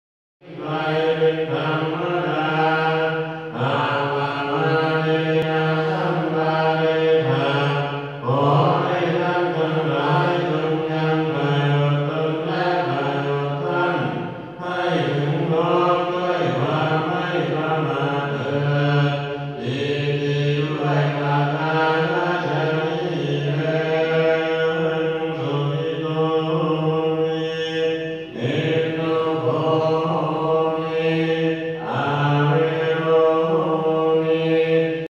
Buddhist chant
This is part of a mediation chant with the Buddhist monks. Peaceful!